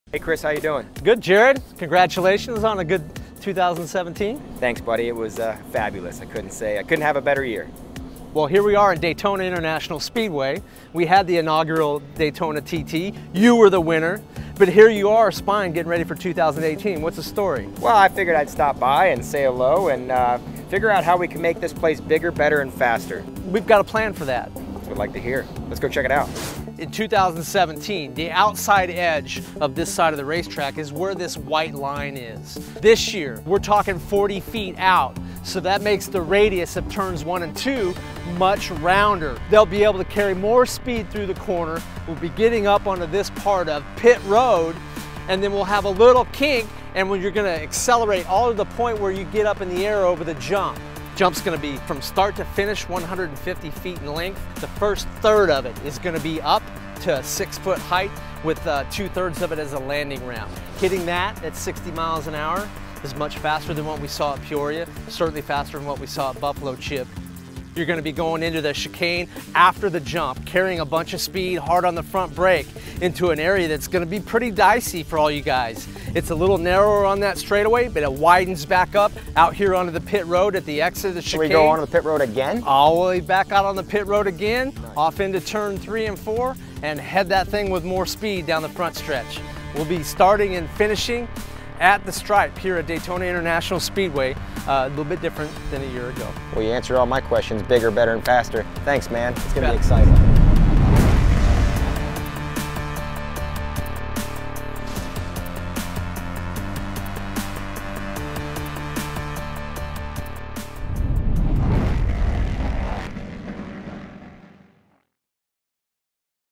2018 DAYTONA TT Track Preview - American Flat Track
American Flat Track's Chris Carr talks about the new track design for the 2018 DAYTONA TT.